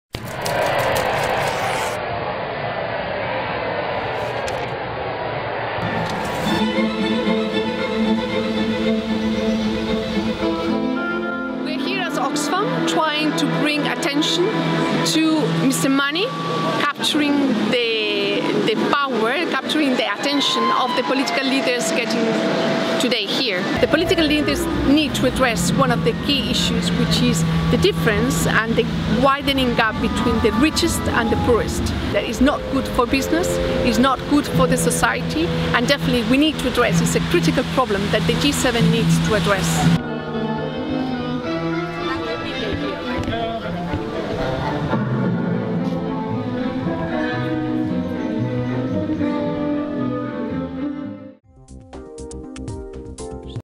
Oxfam Orchestra in G7-minor
Just ahead of the G7 meeting in Brussels, Oxfam urges political leaders to no longer dance to the tunes of Mister Money and address the issue of inequality.